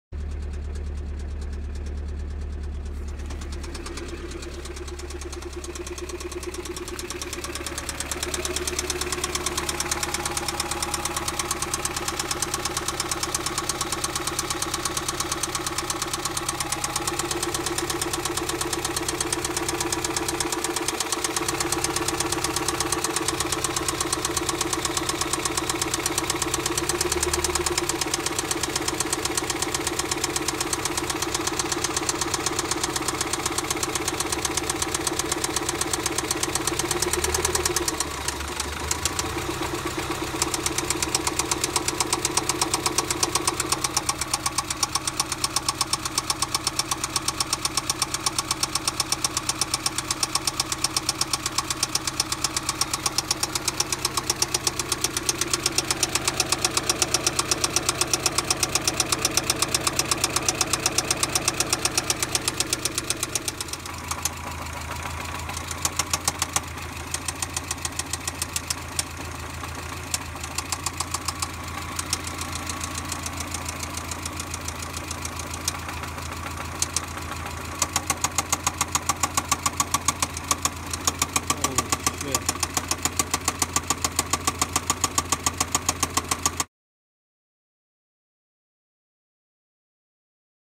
1991 Miata rod knock/spun bearing
200k miles. What seemed to be the common HLA tick turned out to be a spun bearing. If you notice low/no oil pressure, premature oil contamination, rough idle and most importantly a knocking noise, don't waste time hoping it's the HLA tick. It's more than likely a spun bearing if you have all of these symptoms.

If you only notice a ticking noise on startup, start with cleaning the lifters, and then try a thicker oil.